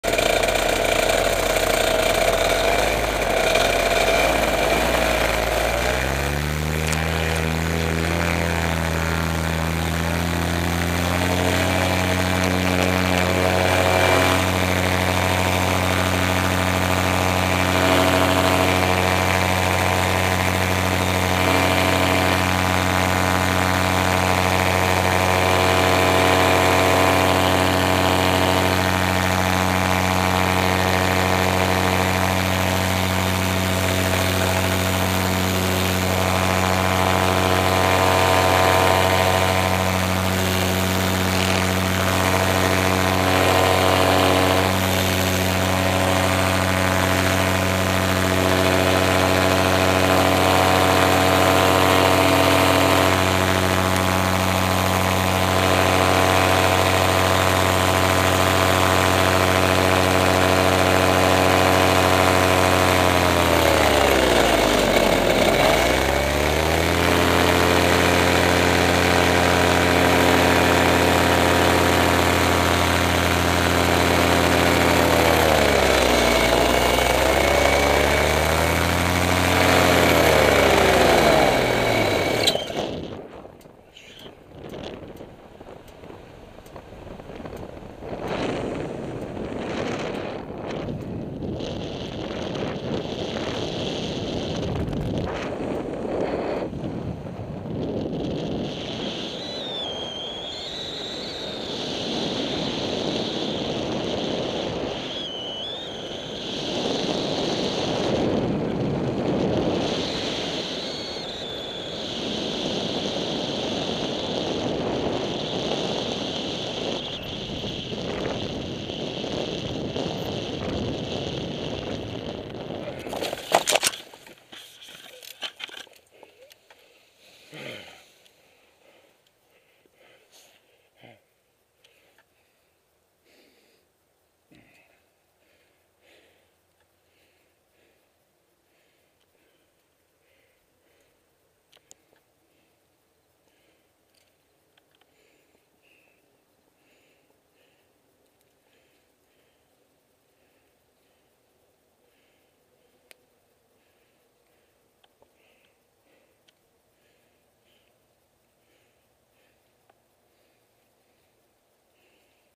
Engine Out Powered Paraglding
On My 7th PPG Flight, the motor quit about 1 minute into the the flight.